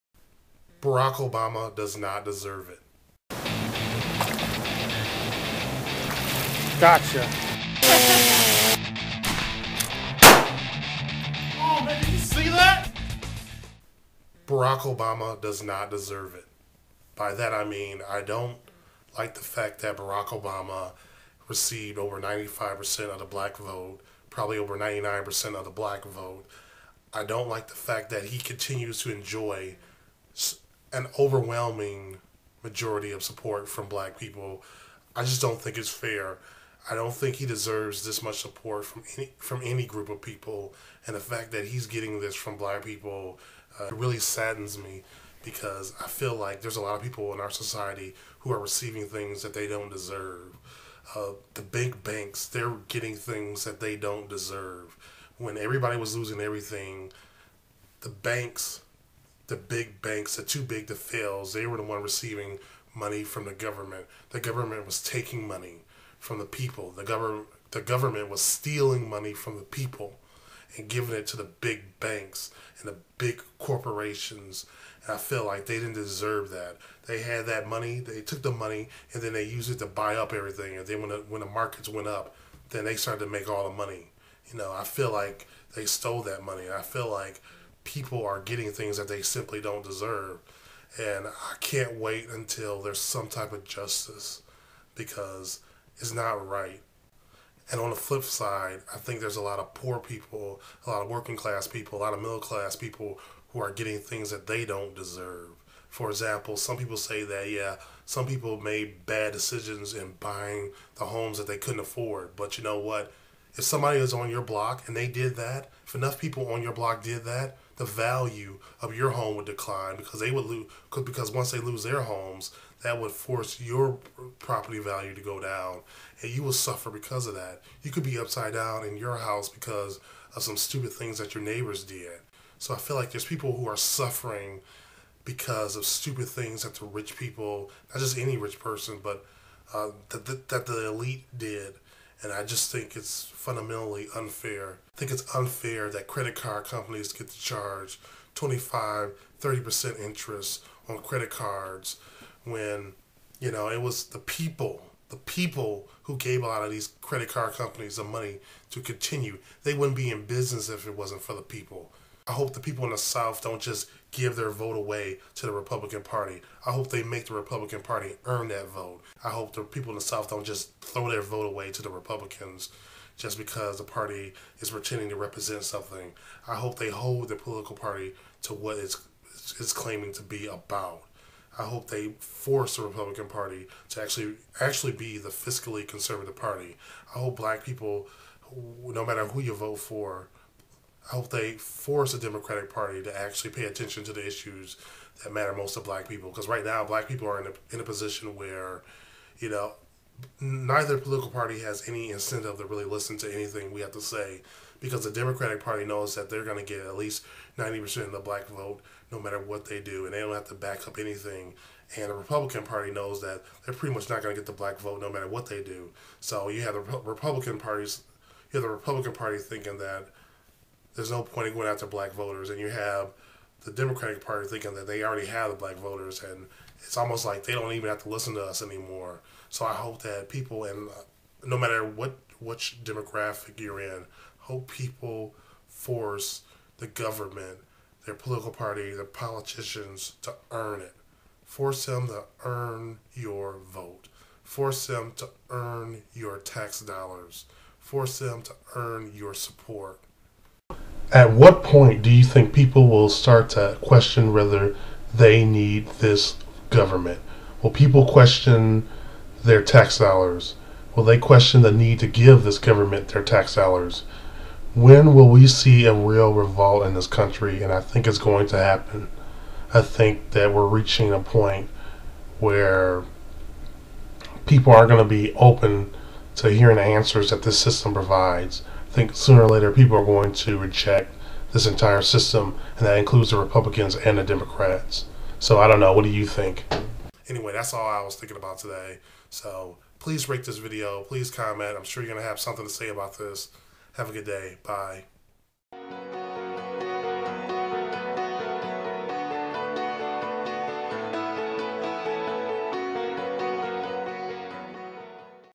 THE GREAT RECESSION: Working People Don't Deserve This Economic Mess
In this video, I talk about the black community's loyalty to Obama and the GREAT RECESSION.

Please support other channels that expose the truth.